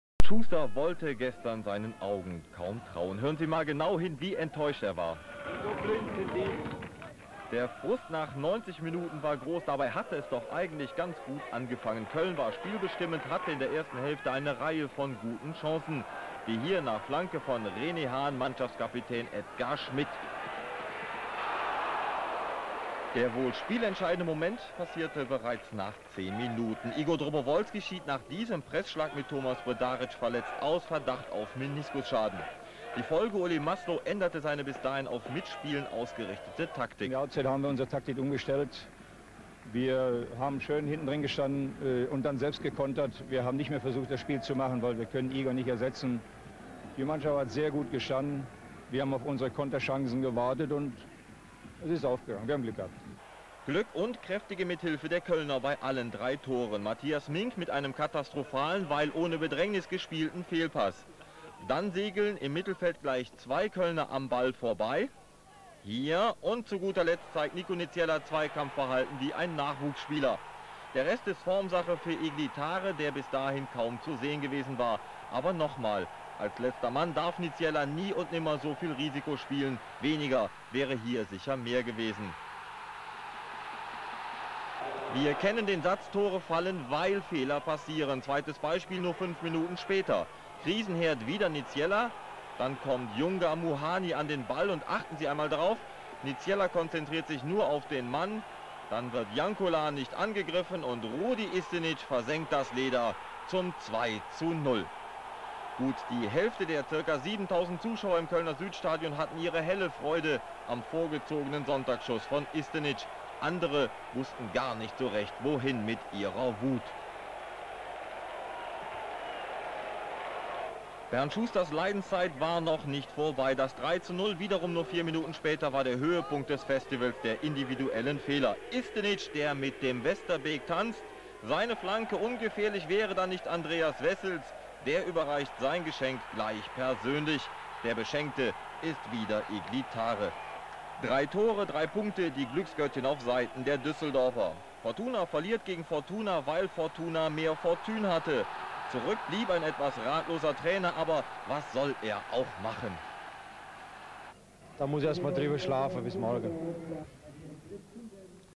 1997 Fortuna Köln - Fortuna Düsseldorf 0:3 | 2x Igli Tare | Interviews Bernd Schuster und Uli Maslo
5.500 Zuschauer im Kölner Südstadion | F95 - Tore:
0:1 Igli Tare, 0:2 Rudi Istenic, 0:3 Igli Tare | Bernd Schuster auf der Kölner Bank, Uli Maslo Trainer für Düsseldorf.